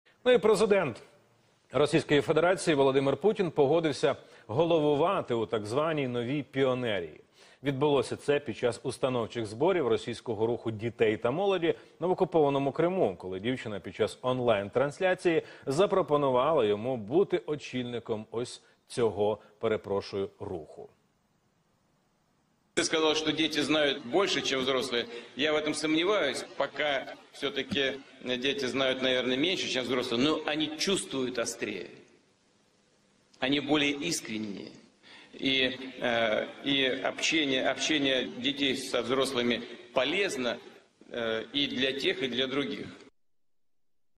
В Росії створили оновлену "піонерію", її очолить Путін
Путін погодився очолити наглядову раду російського руху дітей та молоді.